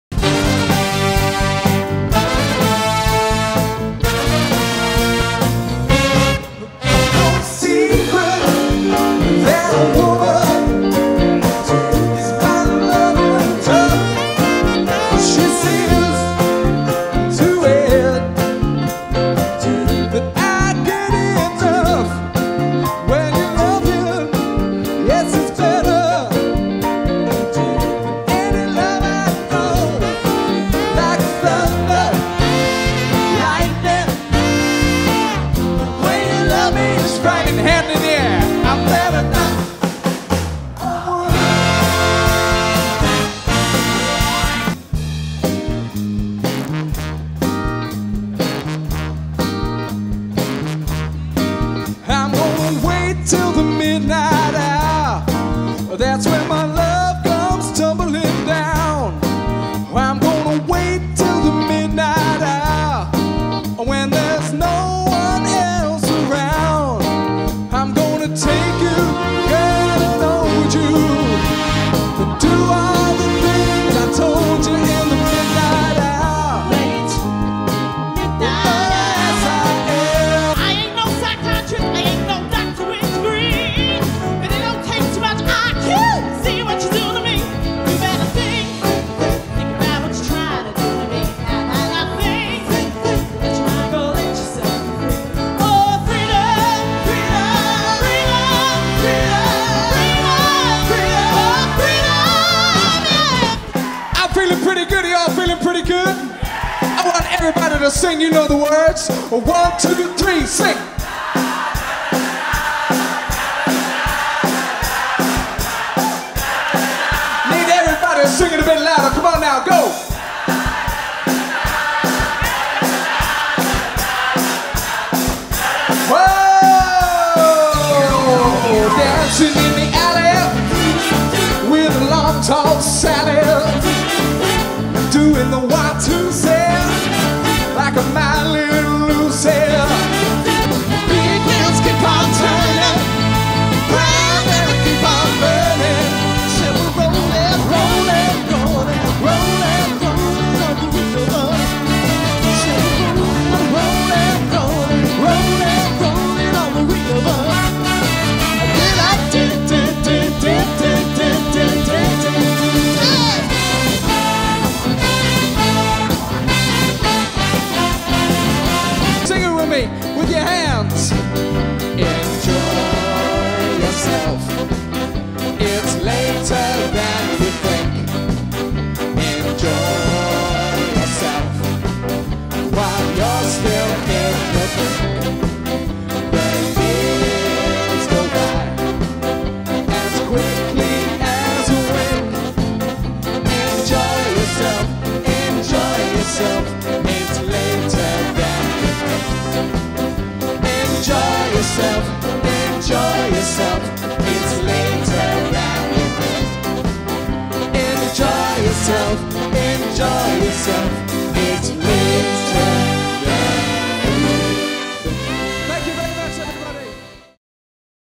SOUL JAM VIDAVEO PROMO
It was great fun filming and editing this Live Event for the sensational Soul Jam.  If you have a live event, musical or otherwise why not get VIDAVEO to film it?  We put out a series of singles from the band over a period of 2 months, which formed a terrific online marketing campaign.